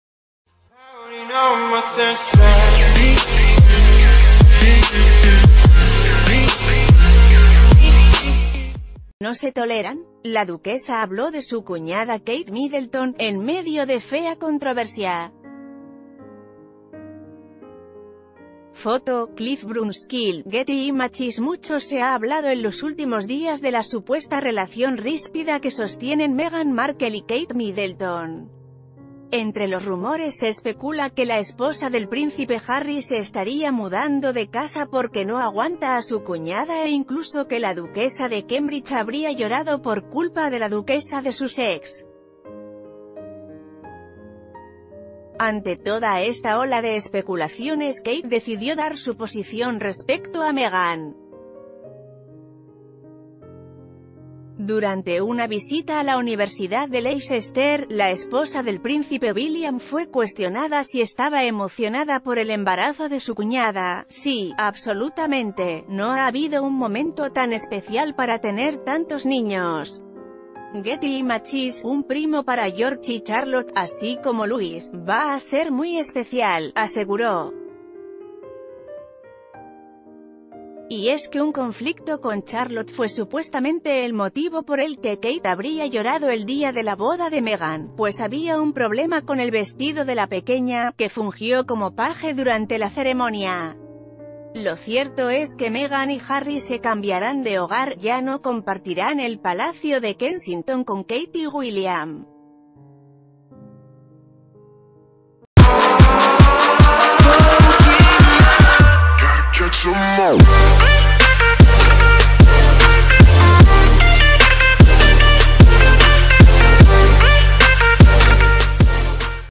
Kate Middleton rompe el silencio sobre supuestos conflictos con Meghan Markle
¿No se toleran? La duquesa habló de su cuñada Kate Middleton, en medio de fea controversia. Foto: Clive Brunskill/Getty Images Mucho se ha hablado en los últimos días de la supuesta relación ríspida que sostienen Meghan Markle y Kate Middleton. Entre los rumores se especula que la esposa del príncip...